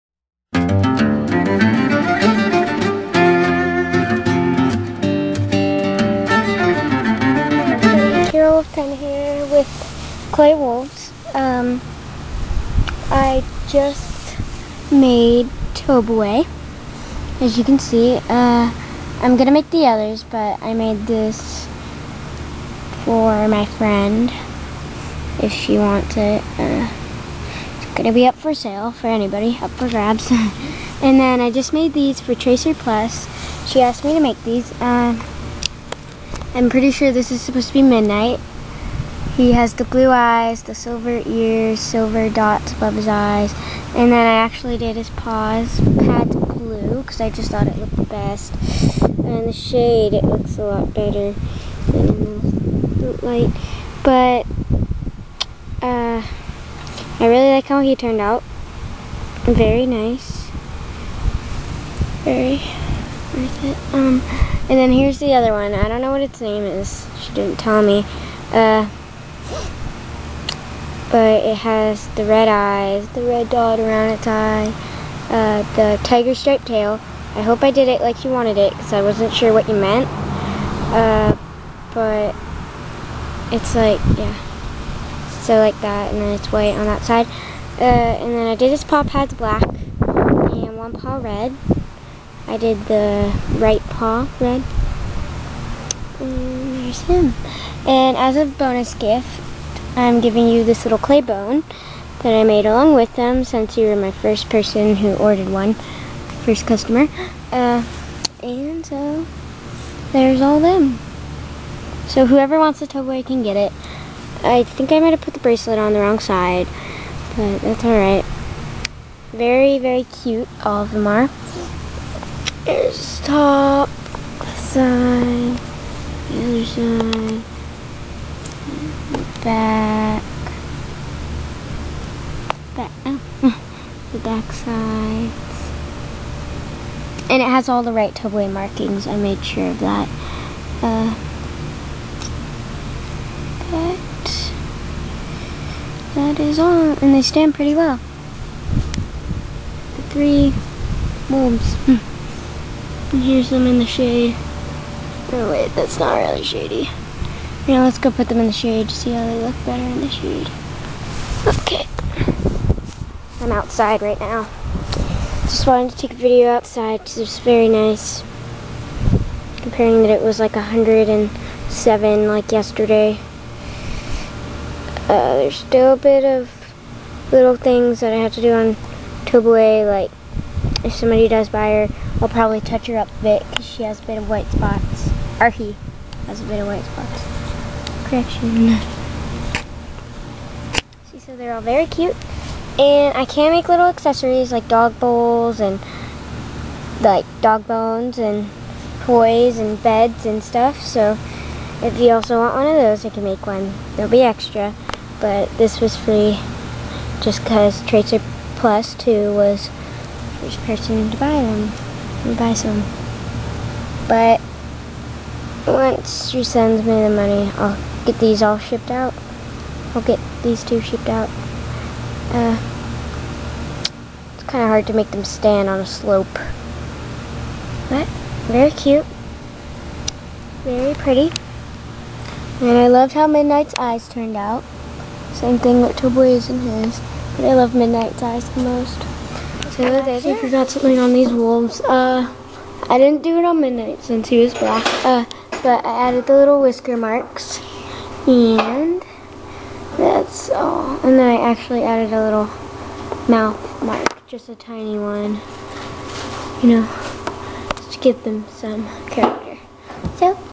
Clay wolves for tracerplus2 And a toboe wolf
here they are tracerplus2 hope you like them! also im sell the tobe to my BFF so if you want one message me i can make another! plz buy a wolf olny $5 for Small and $10 for large +Shipping